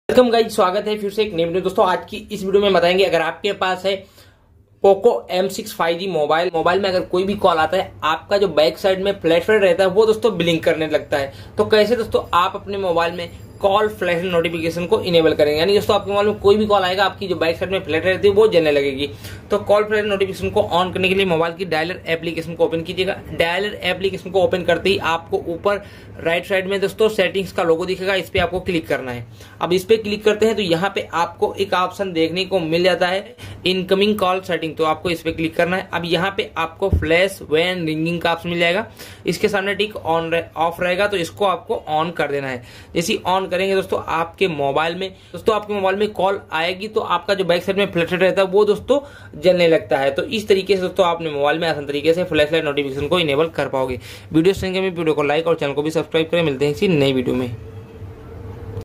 How to Flash on call setting in poco m6 5g || Poco M6 Incoming call flashlight notification on kare
Hello friends Aaj ki Is Video ke madhyam se Maine Aap Logo ko bataya hai ki poco m6 5g mobile me incoming call flash on setting call aate hi flashlight jalegi